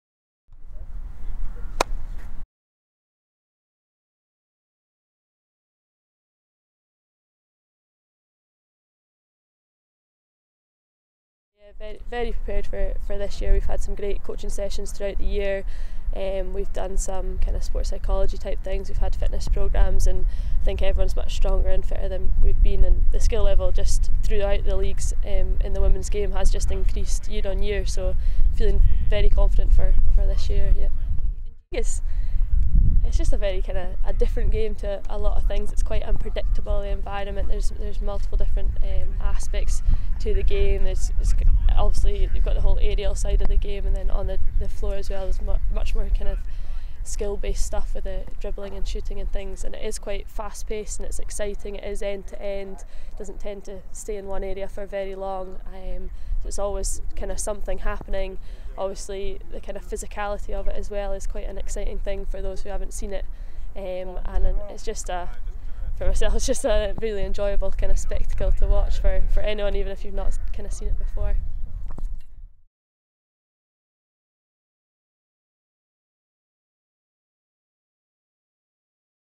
2015 Marine Harvest Shinty Camogie Challenge preview
Scotland Women's Shinty mid-fielder Kirsty Deans talks ahead of the 2015 Marine Harvest Shinty Camogie Challenge.